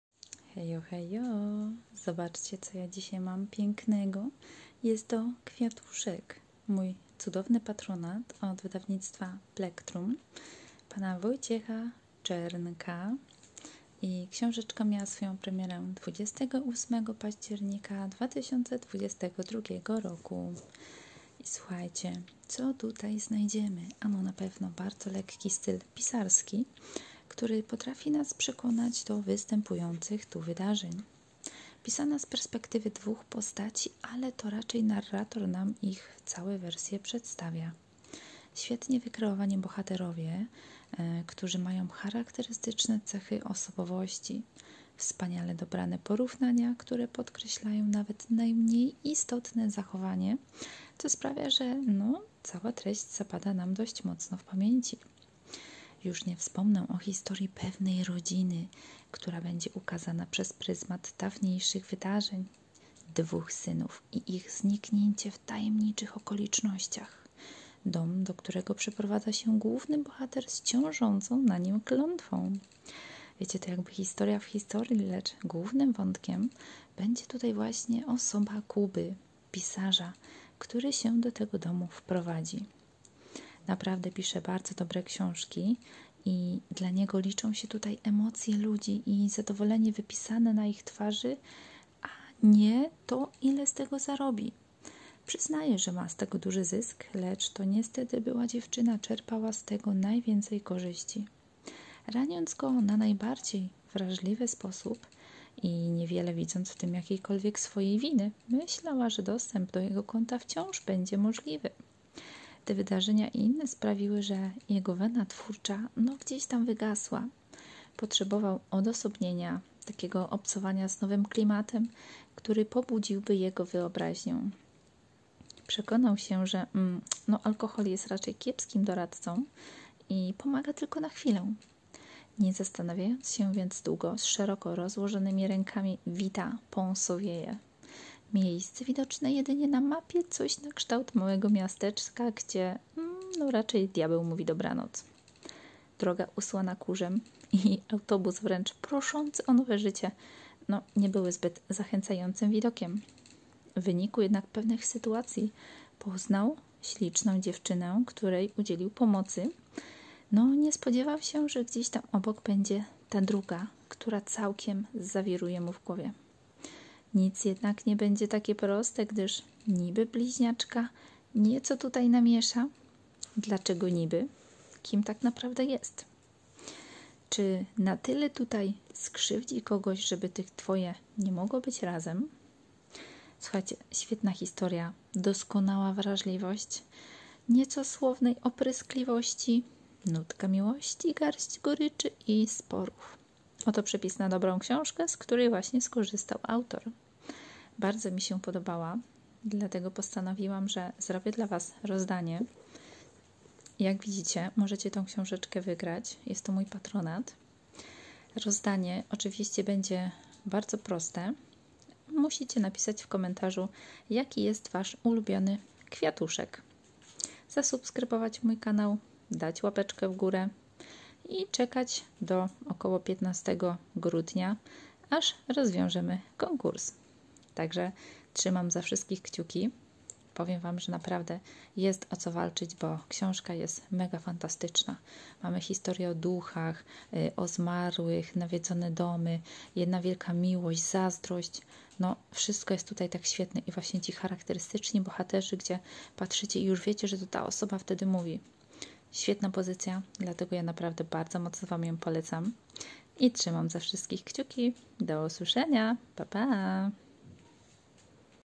Recenzja patronacka rozdanie ,,Kwiatuszek" Wojciech Czernek
Premiera 28 październik 2022r.
Dziękuję Wydawnictwu Plectrum i autorowi za możliwość jej patronowania 💙